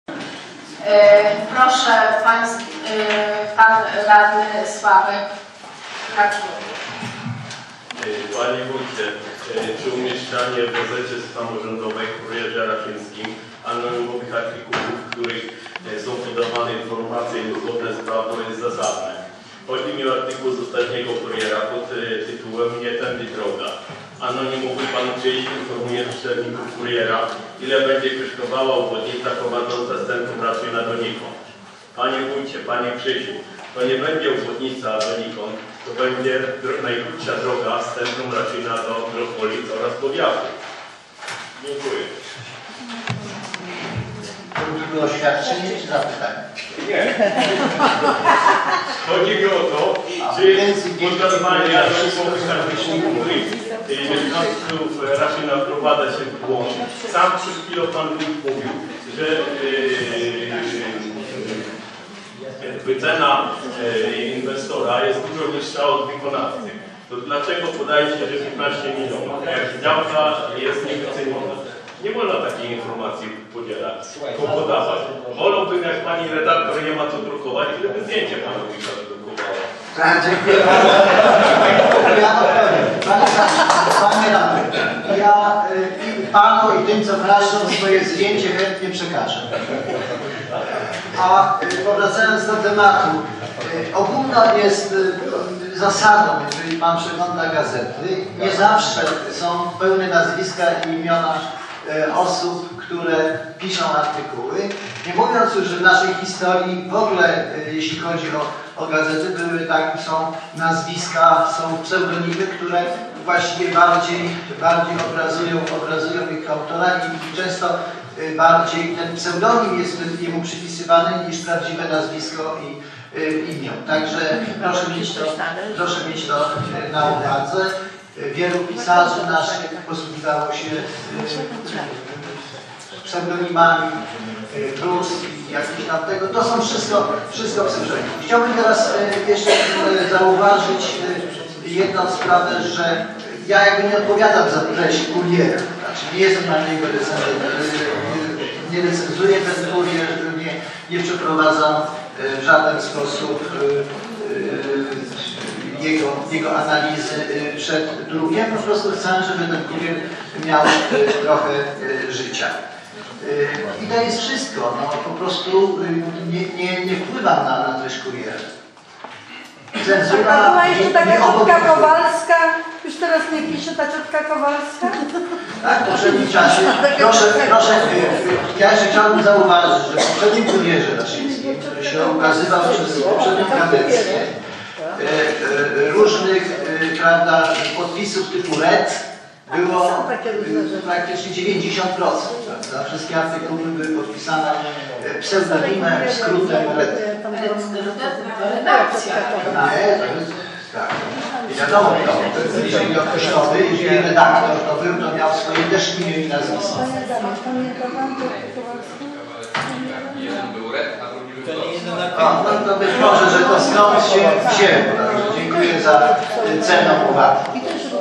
Czy Wójt odpowiada za treść Kuriera?
Radny Sławomir Haczkur:
"Wolałbym, jak Pani Redaktor nie ma co drukować, żeby zdjęcie Pana Wójta wydrukowała"